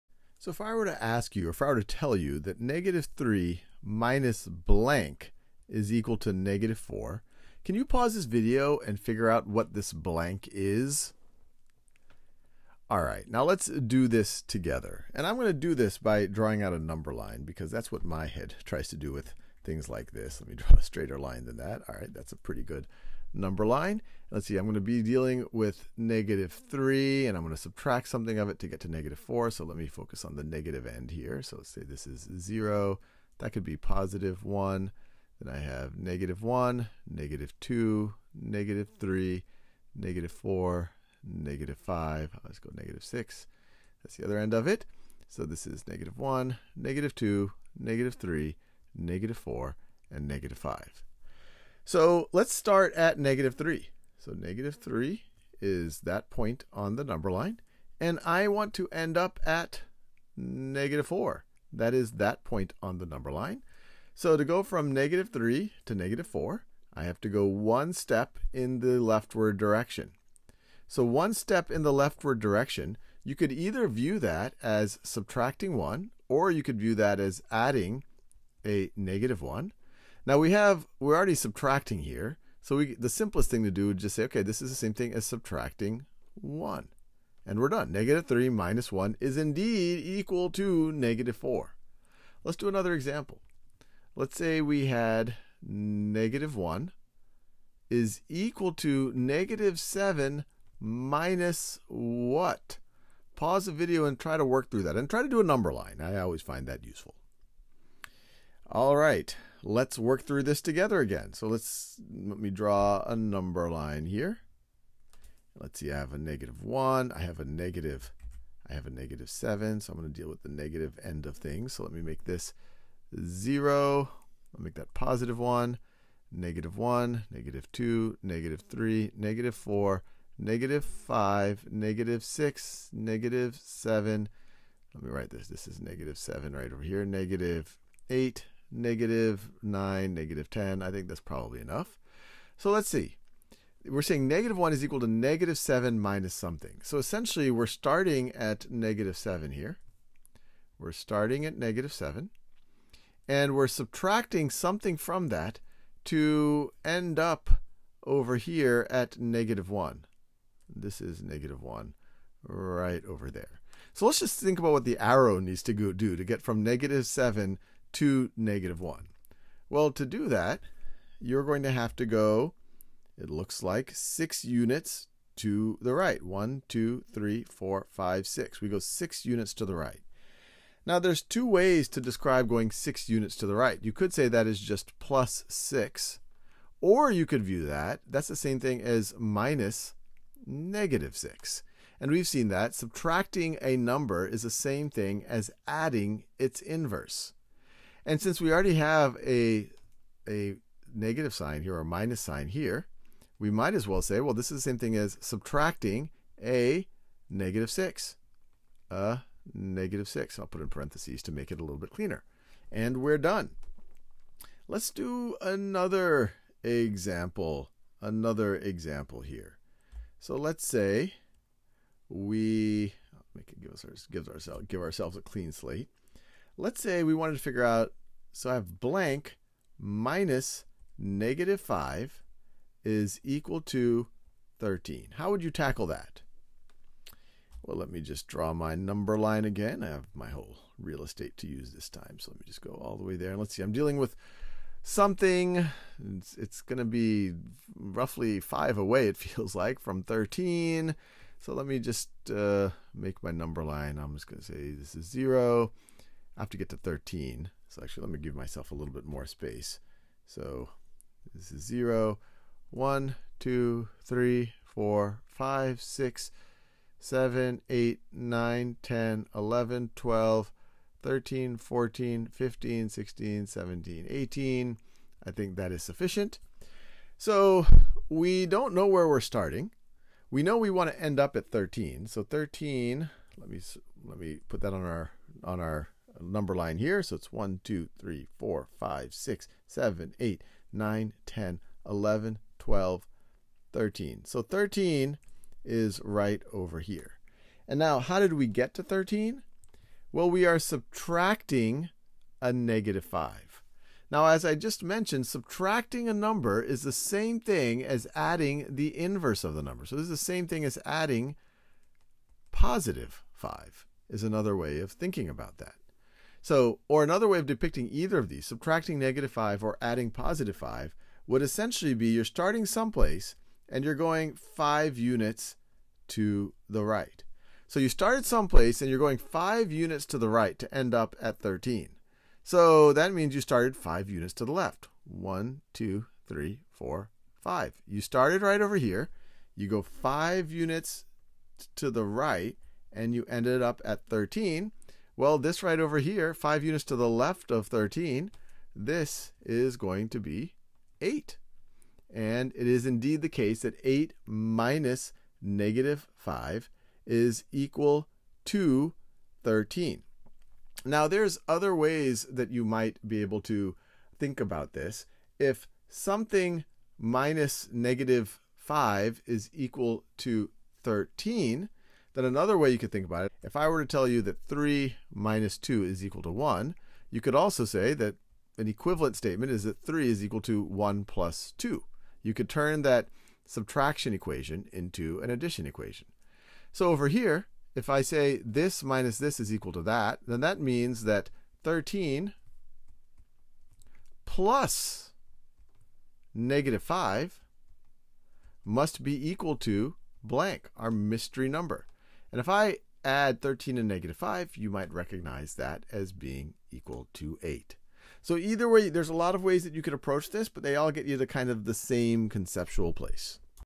Subtracting integers find the missing value | 7th grade | Khan Academy
Courses on Khan Academy are always 100% free. Start practicing—and saving your progress—now!

Use number lines to find missing numbers in subtraction equations with integers. Arrows to the left equivalently mean adding a negative number and subtracting a positive number of the same magnitude. Arrows to the right equivalently mean adding a positive number and subtracting a negative number of the same magnitude.